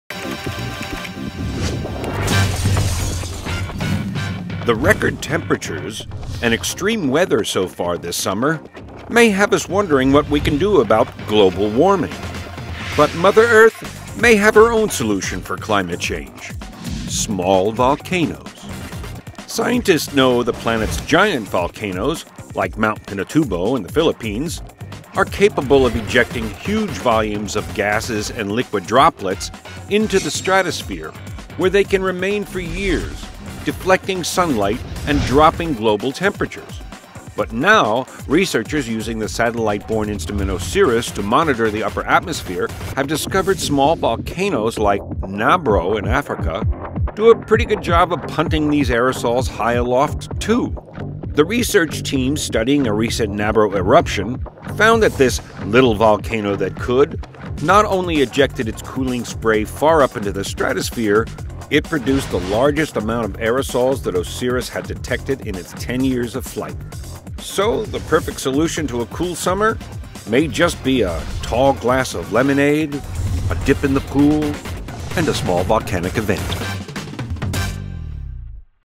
Could Small Volcanoes Slow Global Warming?
The record temperatures and extreme weather so far this summer may have us wondering what we can do about global warming. But Mother Earth may have her own solution for climate change: small volcanoes.

Scientists know the planet's giant volcanoes, like Mt. Pinatubo in the Philippines, are capable of ejecting huge volumes of gases and liquid droplets into the stratosphere, where they can remain for years, deflecting sunlight and dropping global temperatures.
But now, researchers using the satellite-bourn instrument OSIRIS to monitor the upper atmosphere have discovered smaller volcanoes like Nabro in Africa do a pretty good job of punting these aerosols high aloft too. The research team studying a recent Nabro eruption found that this Little Volcano That Could not only ejected its cooling spray far up into the stratosphere, it produced the largest amount of aerosols that OSIRIS had ever detected in its 10 years of flight.

The perfect solution to a cool summer may just be, a tall glass of lemonade, a dip in the pool, and a small volcanic event.